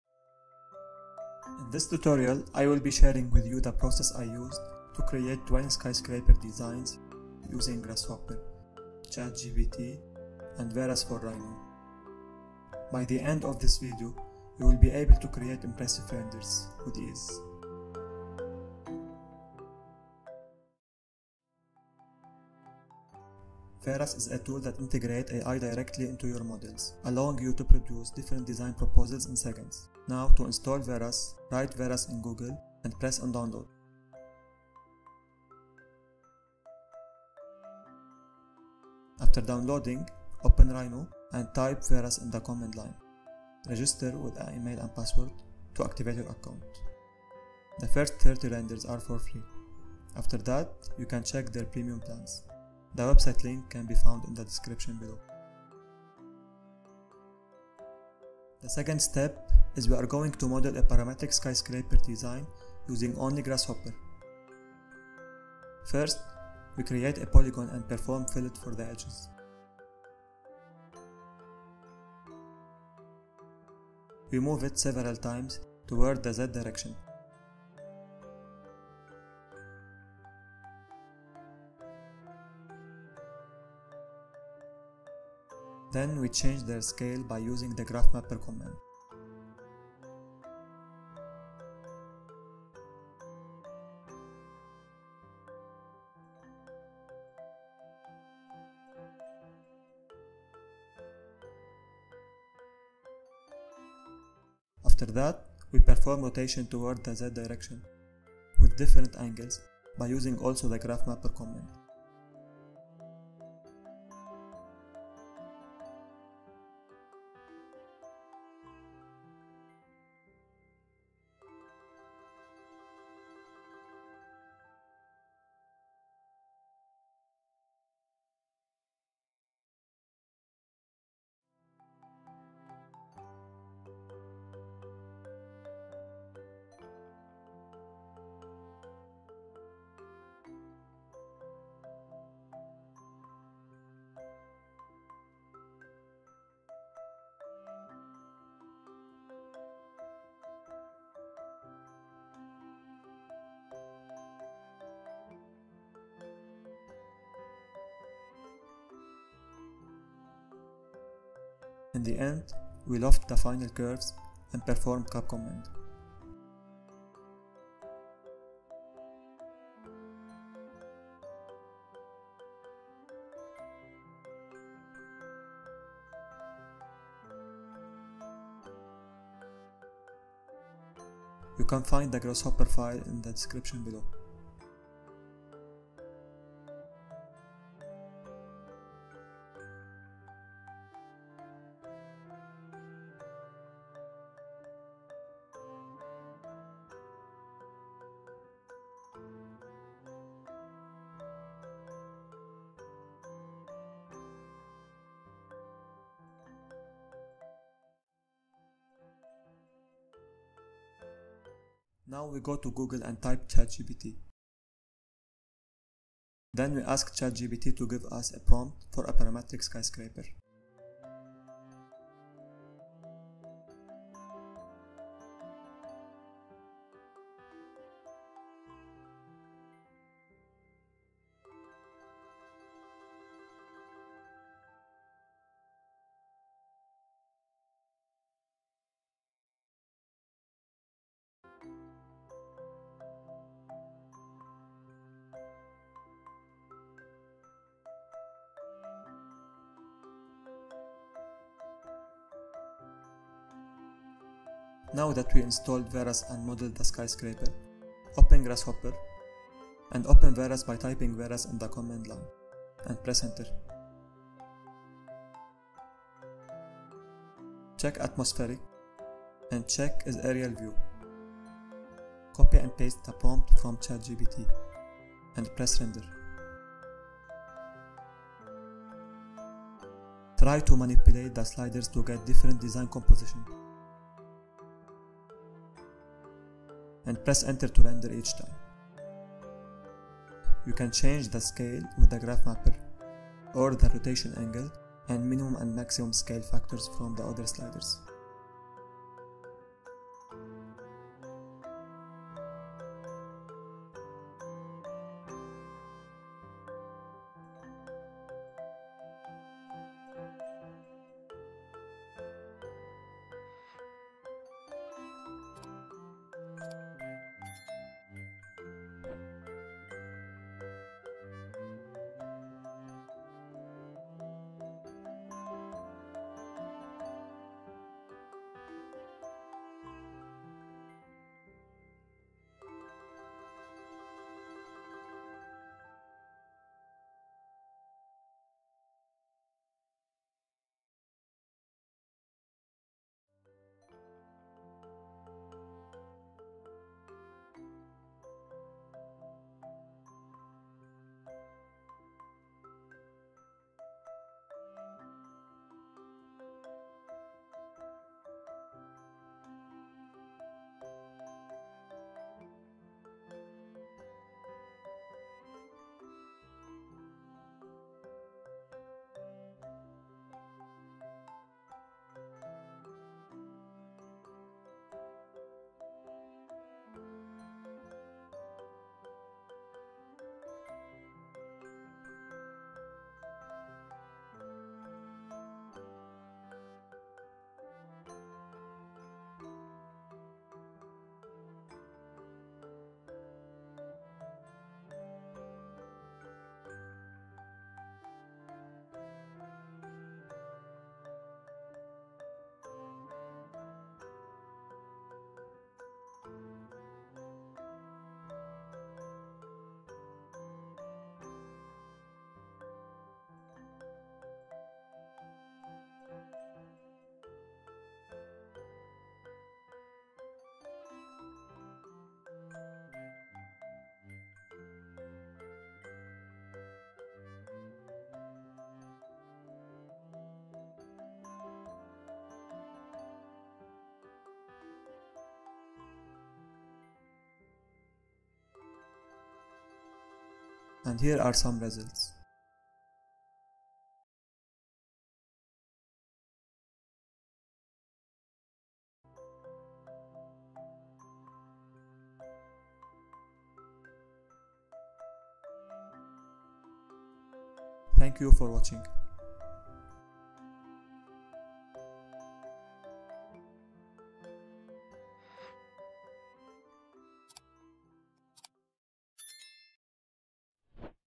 AI-Powered Skyscraper Design: 20 RENDERS in 5 Mins! (VERAS for Rhino)
In this tutorial, I will guide you through the process of using AI to create 20 realistic skyscraper renders in just 5 minutes using the Veras for Rhino AI render engine. We will explore the capabilities of parametric design in Grasshopper, leveraging its input sliders to manipulate the skyscraper design. Additionally, we will utilize ChatGPT, to generate effective prompts for the design process.

To get started, we'll cover the installation steps to set up Veras and activate it within Rhinoceros. Once that's done, we'll delve into modeling a skyscraper design using Grasshopper. By adjusting the input sliders, we'll be able to experiment with different design proposals.
Finally, we'll demonstrate how Veras for Rhino can render these designs quickly and realistically.

Start using AI to create impressive renders for your architectural projects in no time.

Timecodes:
0:00 Introduction
0:20 Installing Veras
0:55 Modeling in Grasshopper
3:28 Prompt from Chatgbt
4:12 Trying Some Prompts
7:17 Final Results

AI Rendering Tutorial, Parametric Architecture, Rhino tutorial for designers, Grasshopper 3D tutorial.